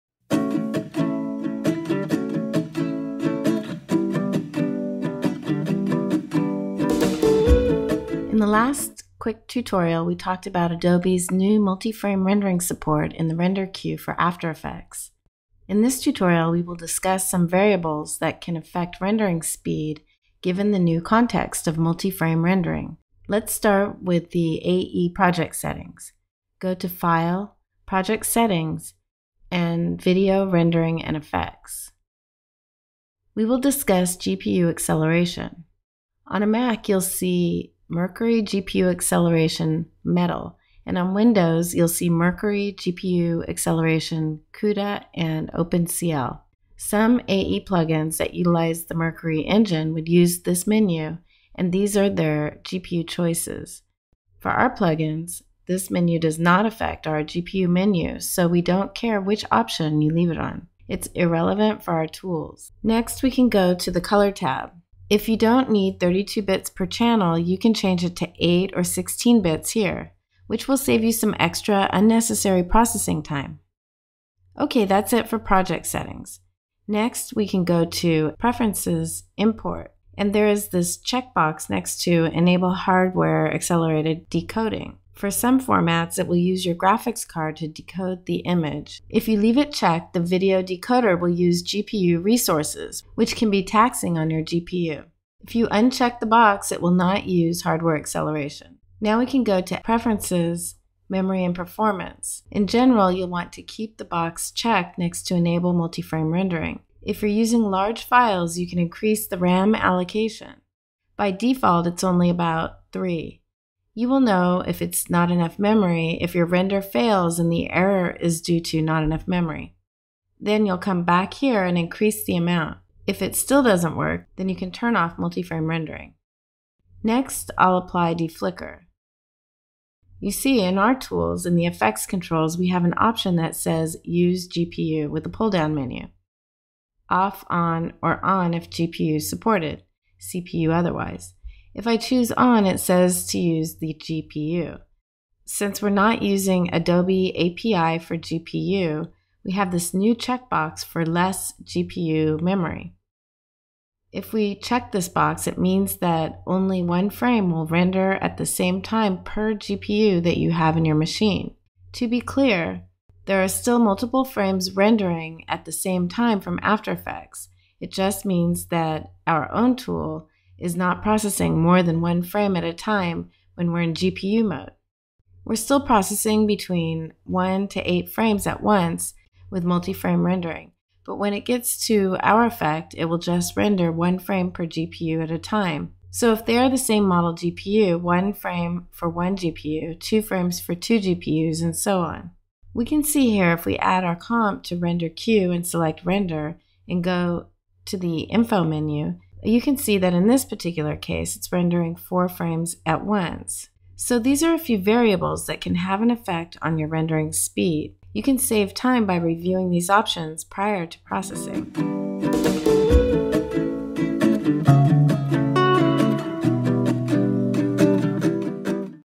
Multi-Frame Rendering - Variables that affect processing time
In this tutorial, we discuss some variables that can affect rendering speed given the new context of Multi-Frame Rendering.

**This tutorial uses After Effects 18 public beta, the methods we describe are subject to change.

**Note: Known issue, if you see artifacts on certain systems with dual GPU, note that to benefit from 2 or more GPU with our tools you might need to turn off the Hardware acceleration checkbox in Import Preferences. Also Note: Preferences are not carried over with project settings if you render on a different machine.